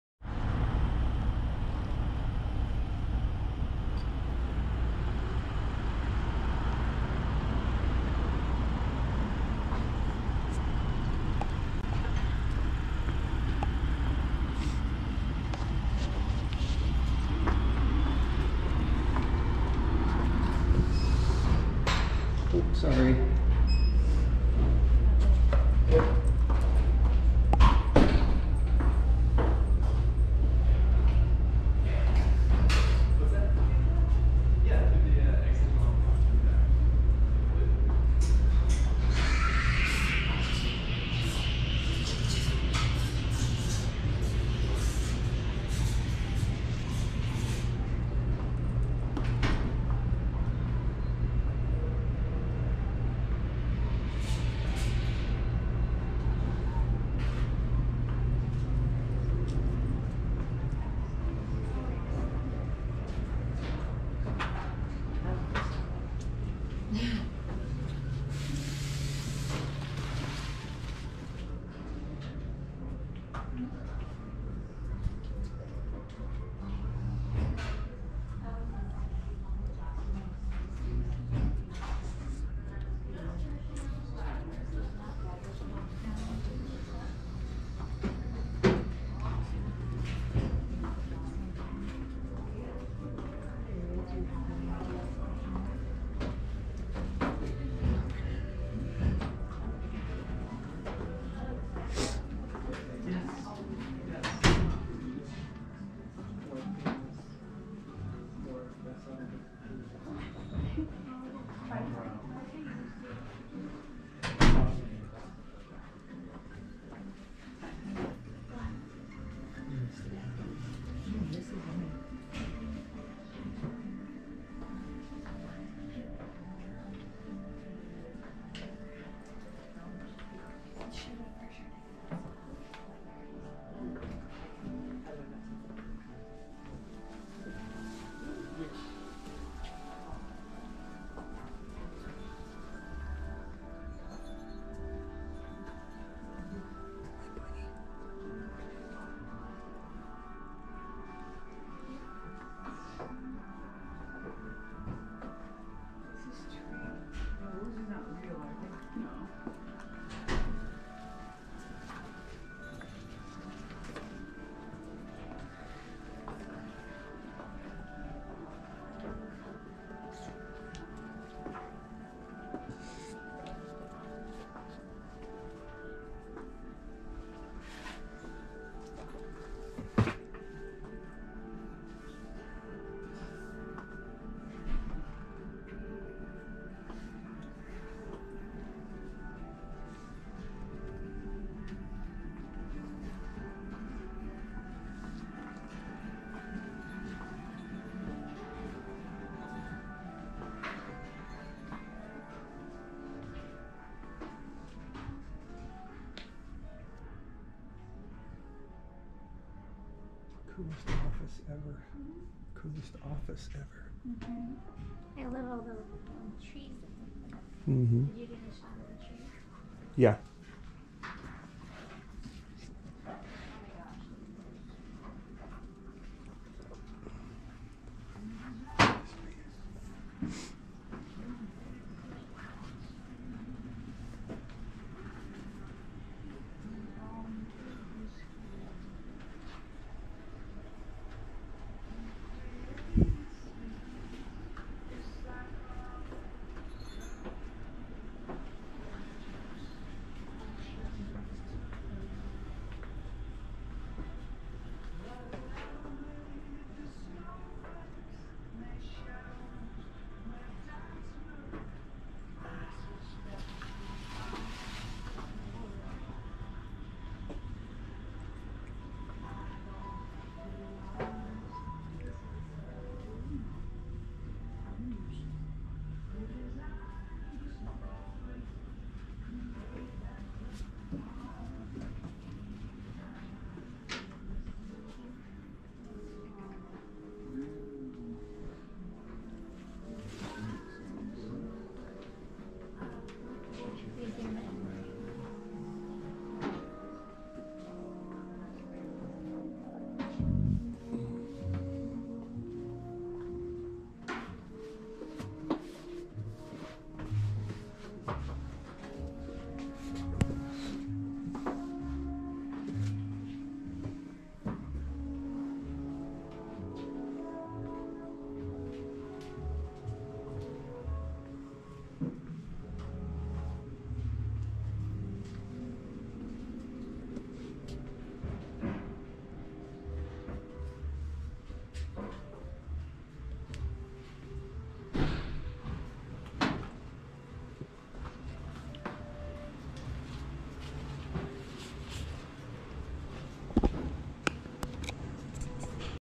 Las Vegas Walking Tour (The Writer's Block Book Shop)
This is a video walking tour of The Writer's Block Book Shop in downtown Las Vegas, Nevada. Billed as a "bookshop, coffee shop, young writers' workshop and artificial bird sanctuary in Downtown Las Vegas," it is an independent bookstore offering free creative-writing classes to grades K-12. The coffee shops offers espressos, lattes, cappuccinos, hot or iced teas, smoothies and other specialty drinks, and many sweet and savory snacks (gluten free and vegan options available). The bookstore itself is a quirky, fun, and creative space full of whimsical artifacts--they even have a live bunny! This is BY FAR my favorite book shop in all of Las Vegas. If you want to spend some quiet time off the beaten Vegas track, I highly recommend The Writer's Block. I appreciate you!